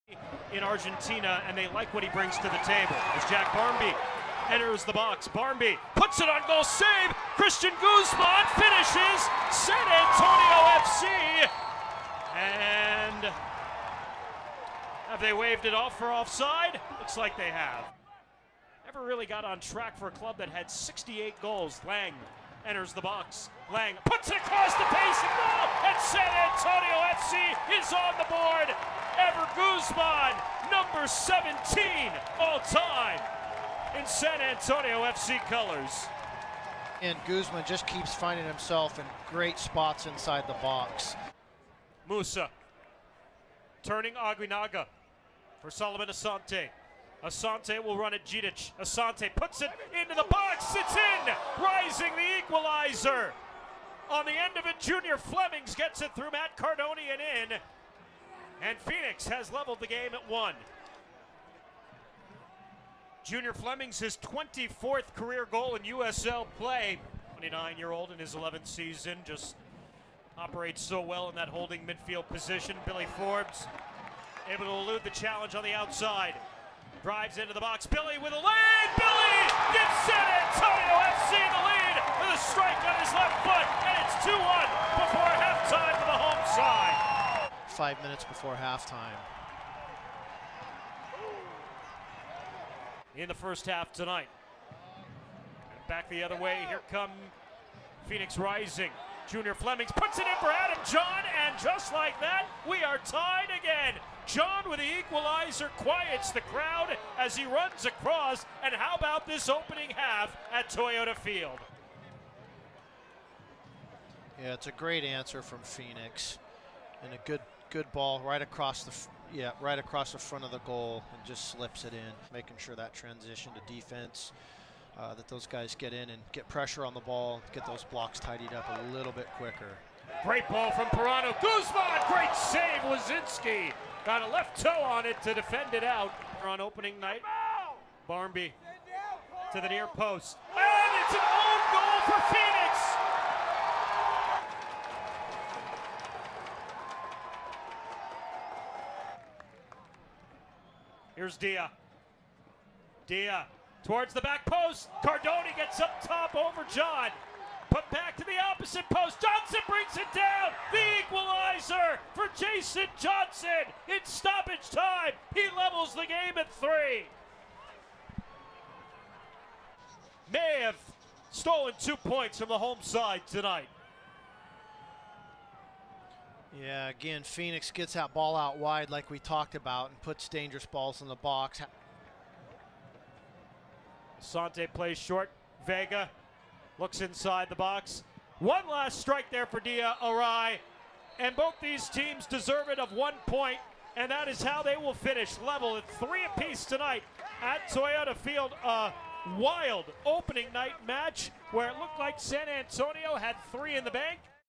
HIGHLIGHTS: San Antonio FC 3-3 Phoenix Rising FC 3.10.19
Phoenix Rising trailed three times on the road in its season opener, but the club equalized each time and earned a hard-fought point in a 3-3 draw against San Antonio FC. Phoenix Rising debutants Junior Flemmings and Adam Jahn notched goals in their club debuts while Jason Johnson scored the equalizer against his former club in stoppage time.